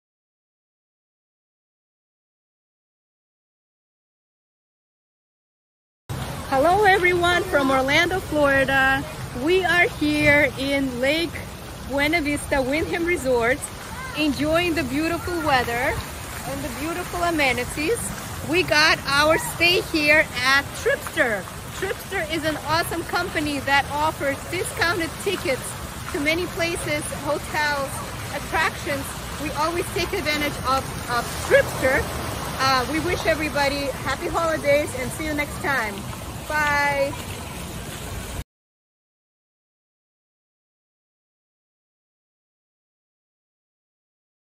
Julie’s Relaxing Getaway at Lake Buena Vista Resorts - Thanks Tripster! 🏖️🌟✨
🌟 Join Julie as she enjoys her fantastic day at Lake Buena Vista Resort in Orlando, Florida! 🌟 She’s having an incredible time relaxing and exploring the resort, all thanks to Tripster’s fantastic ticket deals! We’re so happy to see Julie enjoying her adventure and can’t wait to see where her next journey takes her. Thanks for sharing your experience, Julie! 🥳✨